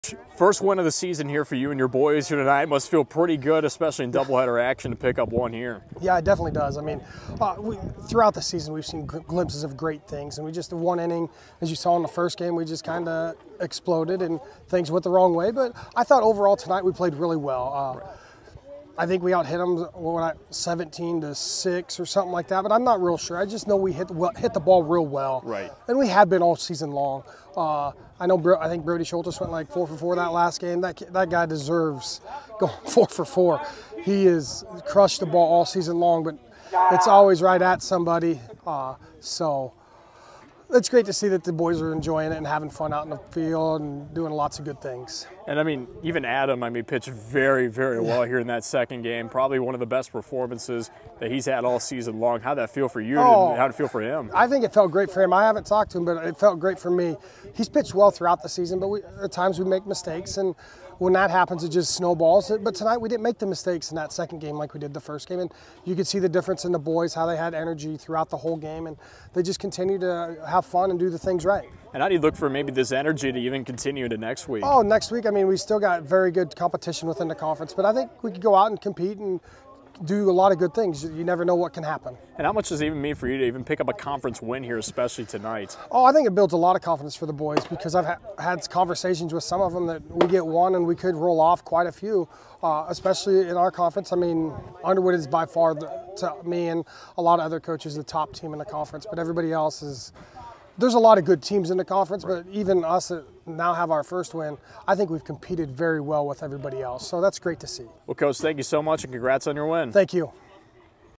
Audubon Head Coach Eric Borkowski on First Win of the Season
Audubon was able to take game #2 of the doubleheader to win the competition 12-2 to earn their first win of the season. He talked with Audubon head Coach Eric Borkowski to get the inside look.